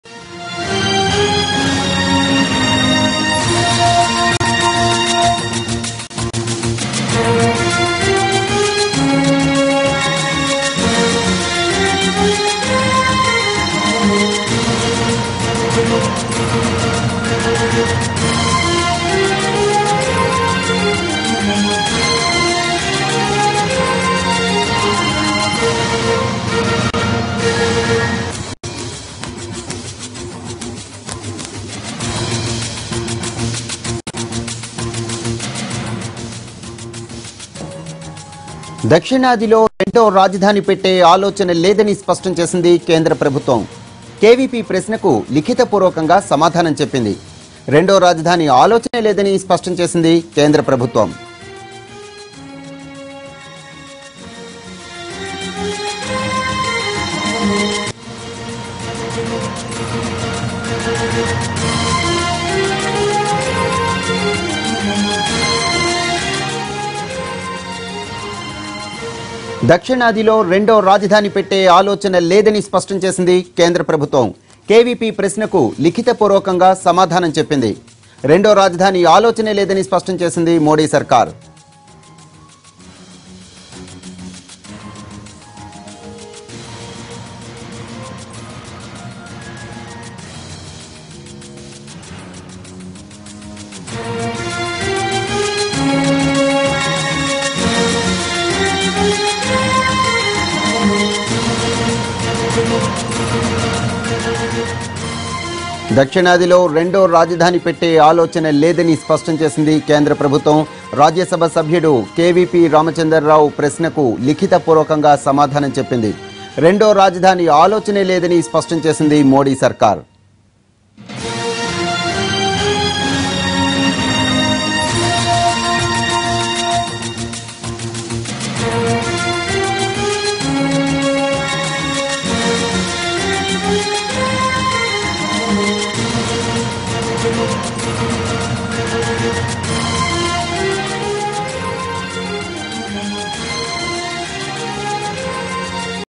Central Govt Gives Clarification on 2nd Capital in South India | NTV
Central Govt Gives Clarification on 2nd Capital in South India